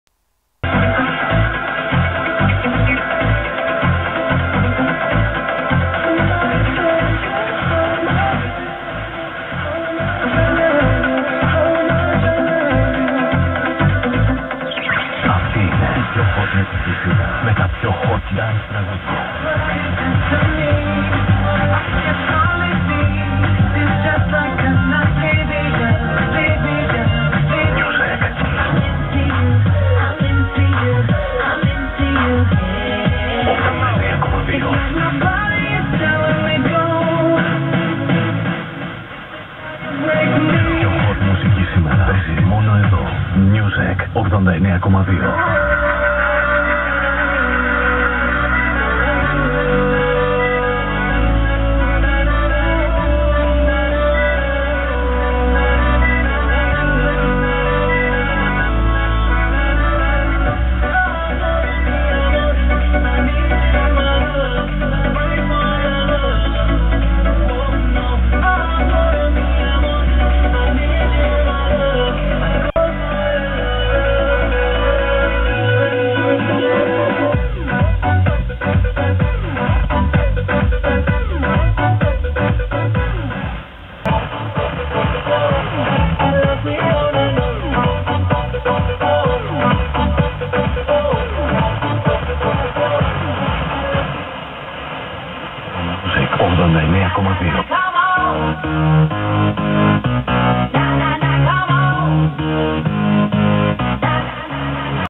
Sporadic-E: Music 89.2 from Athens received in Bochum, Germany
received on 27th July 2011, Sporadic-E reception